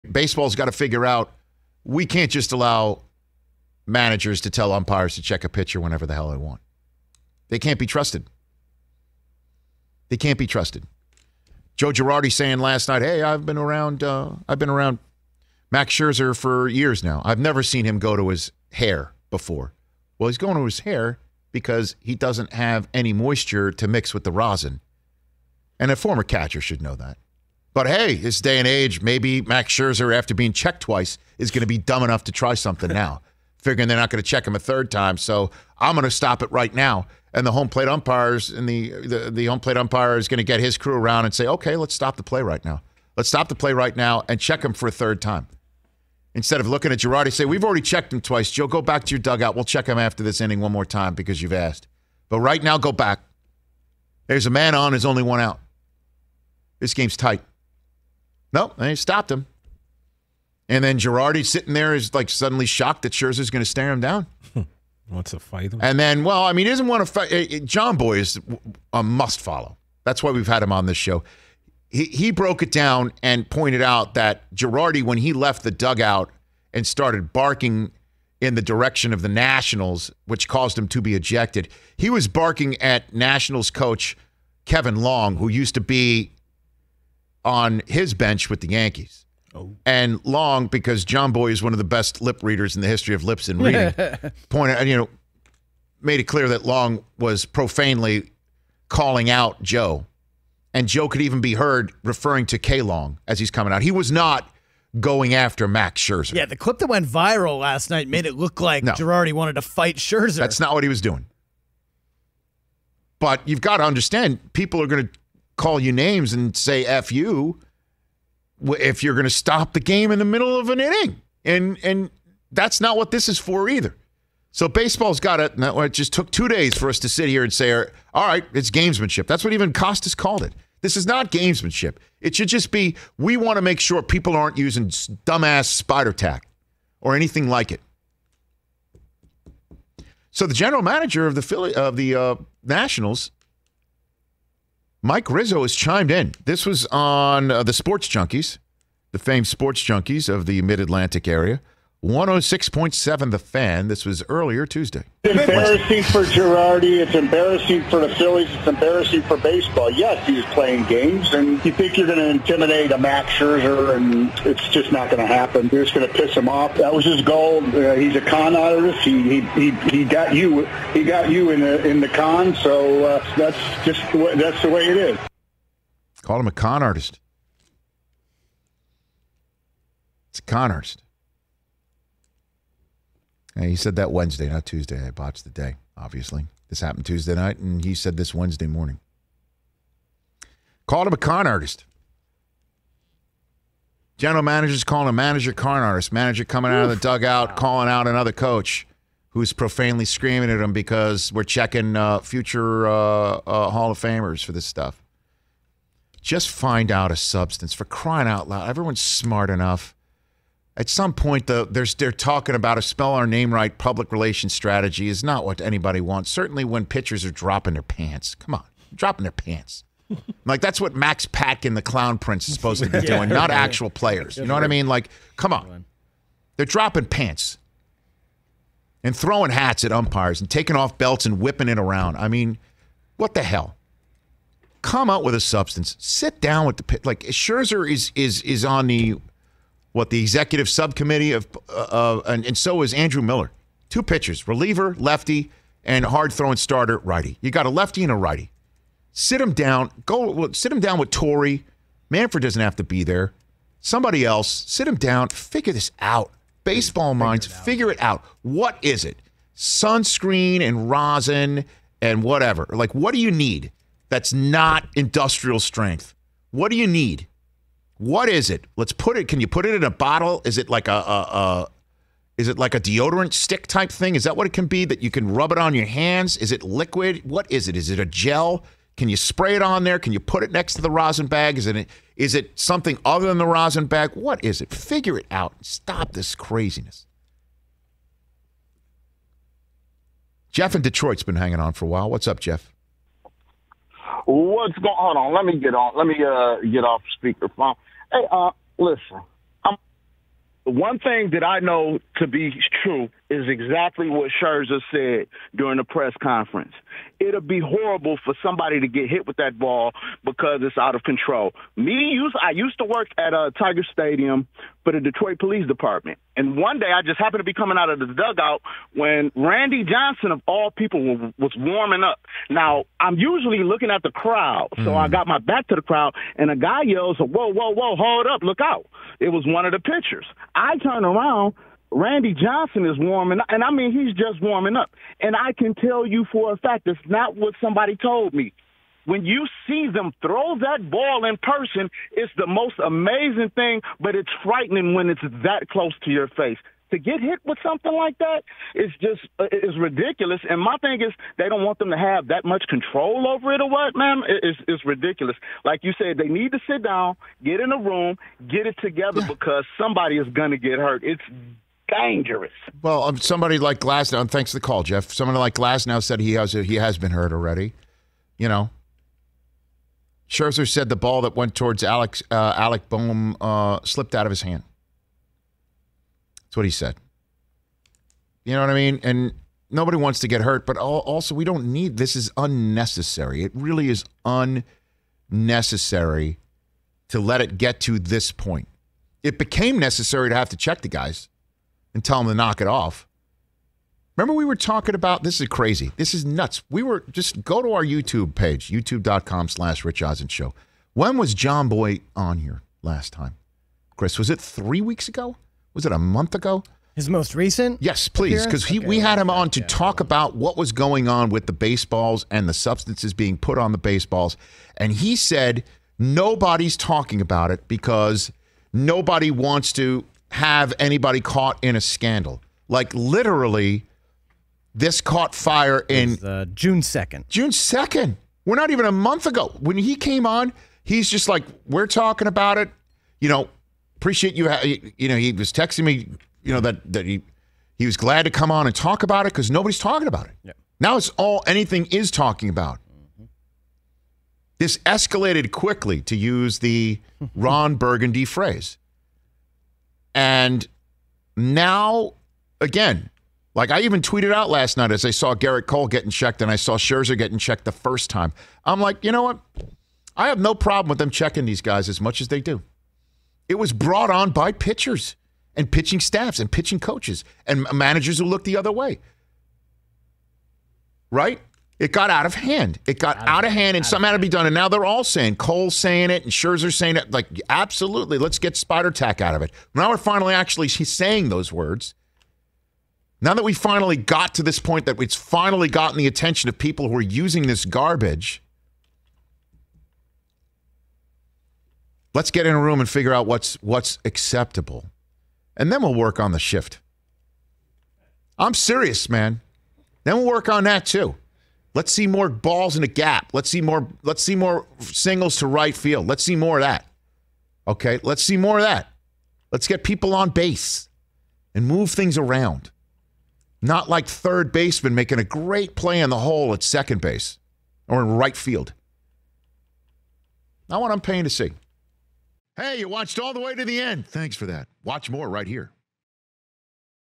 “They Can’t Be Trusted” - Rich Eisen on MLB Managers’ Gamesmanship Misuse of Foreign Substance Rules
Rich Eisen rails against managers manipulating MLB’s new rules enforcement on pitchers using foreign substances on the mound for the wrong reasons.